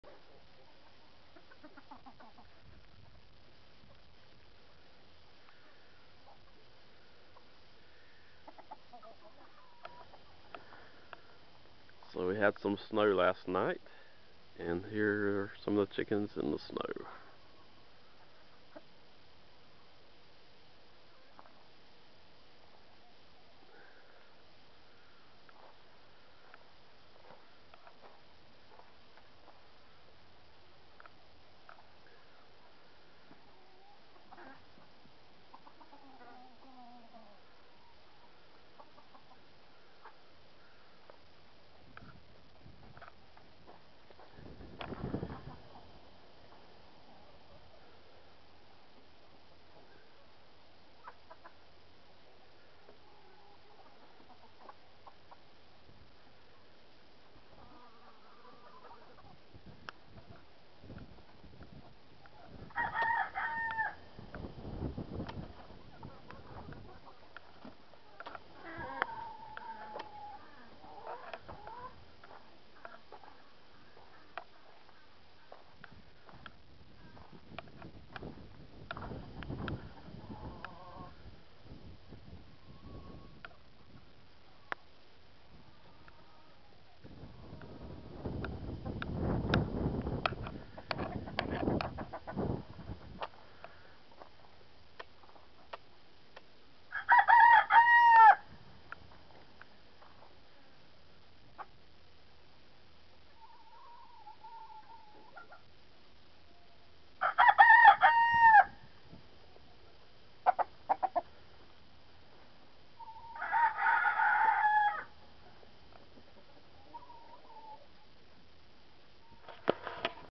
South Carolina chickens in the snow
We had a little snow last night. Actually for us it was quite a bit considering we rarely get snow here. The chickens didn't seem to mind too much.